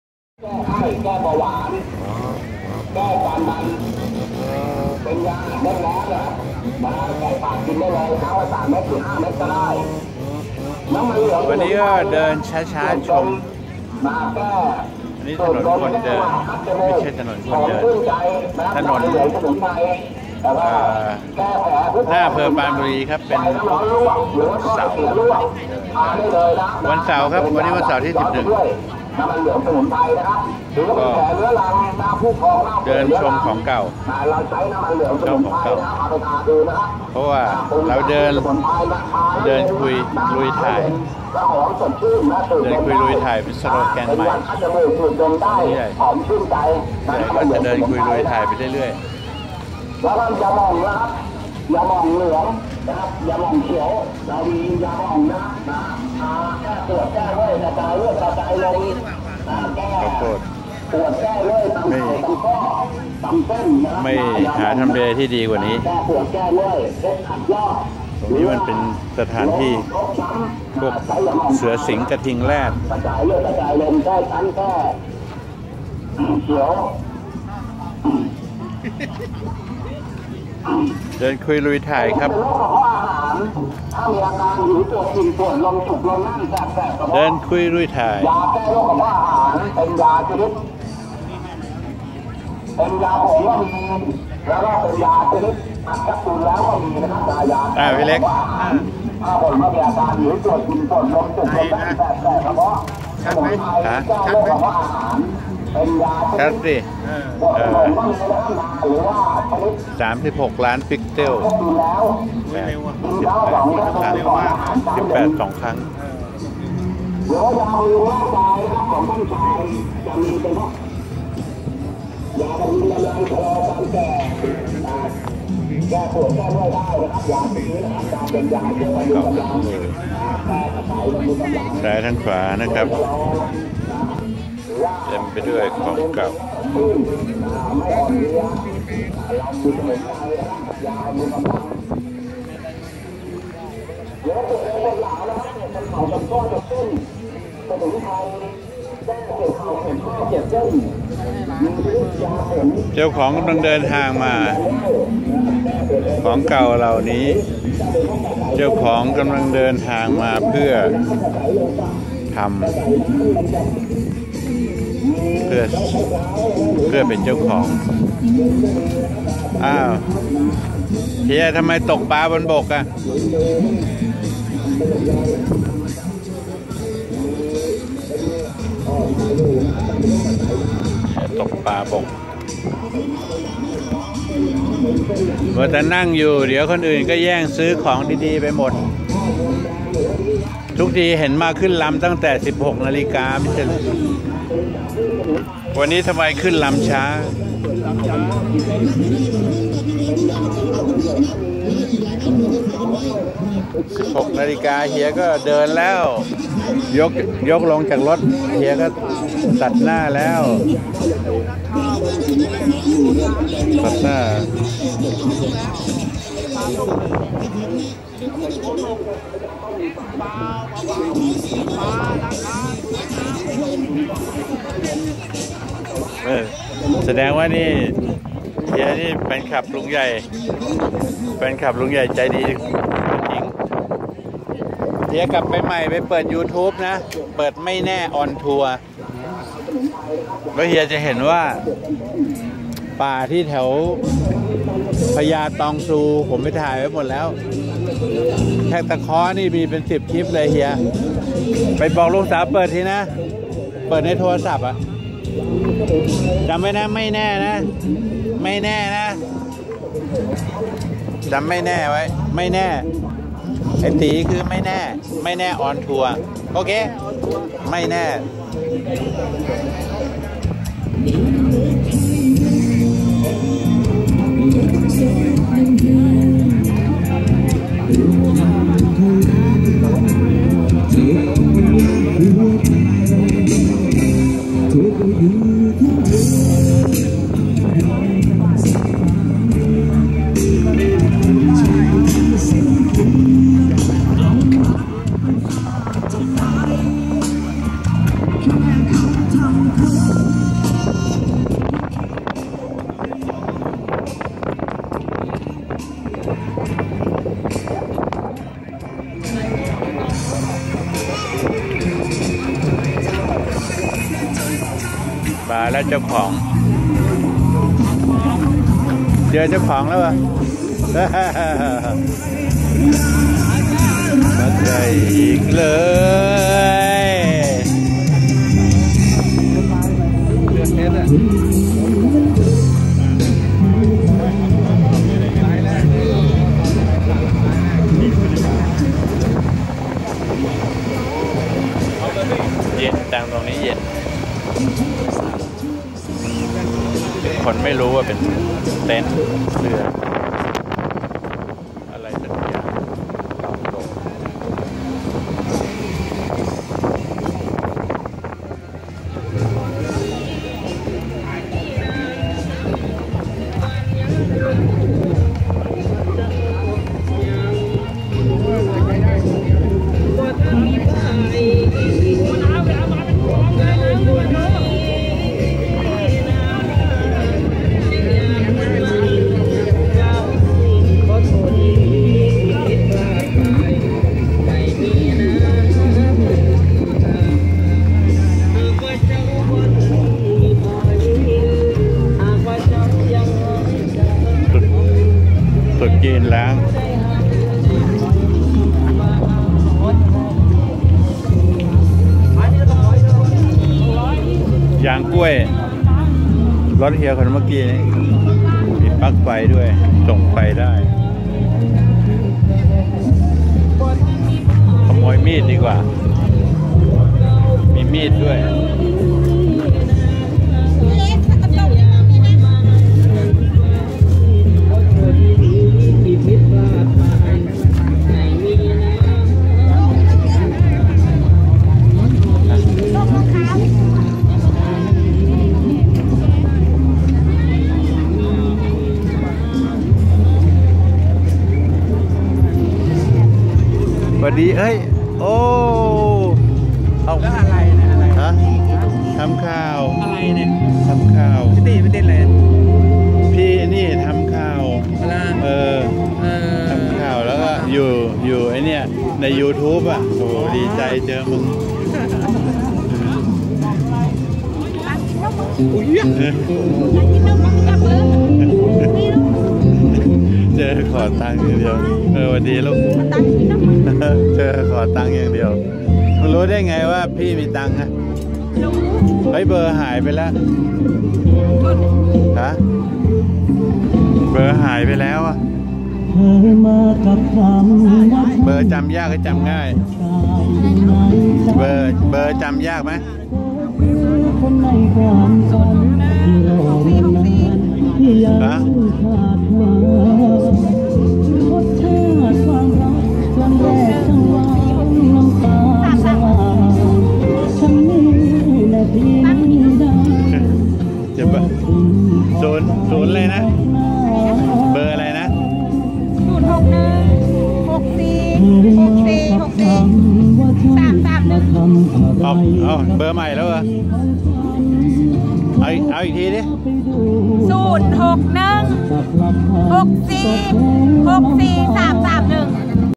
ตลาดเปิดท้ายหน้าอำเภอปราณบุรีวันนี้เป็นอย่างไร
ทุกทุกวันเสาร์ตลาดเปิดท้ายของเสี่ยเล็กหลังปรานพลาซ่าตรงข้ามอำเภอปราณบุรีครับ